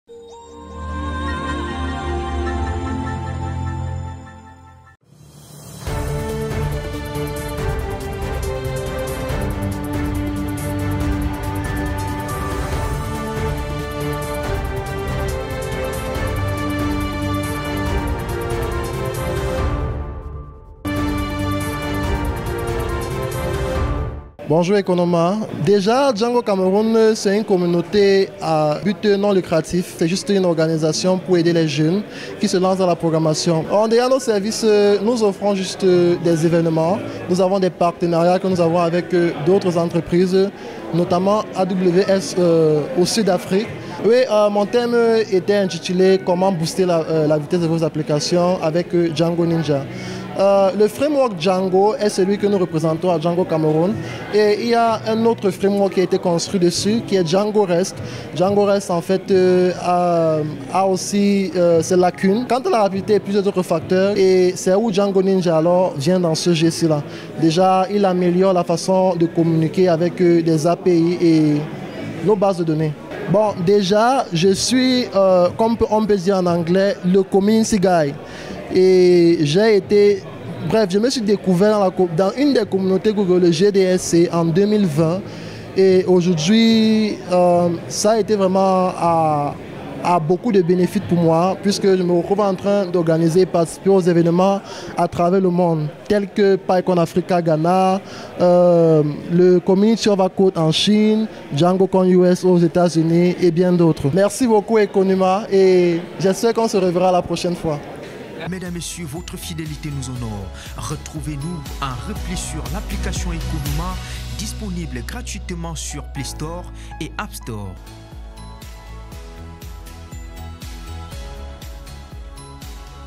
Stève Yonkeu présente Django Ninja au DEVFEST Yaoundé 2024 !
Découvrez Django Ninja, un framework innovant qui optimise les performances des applications Django, présenté par Stève Yonkeu, membre actif de Django Cameroun. Dans cette vidéo, apprenez comment ce framework surpasse Django REST Framework en termes de rapidité et d'efficacité, et découvrez comment Django Ninja améliore la communication avec les API et les bases de données. Stève partage son expérience au sein des communautés Google GDSC et son expertise acquise lors d'événements internationaux tels que pCon Africa au Ghana et Community co en Chine, ainsi que DjangoCon US, tout en présentant Django Cameroun, une organisation qui forme les jeunes développeurs avec le soutien de partenaires majeurs comme AWS.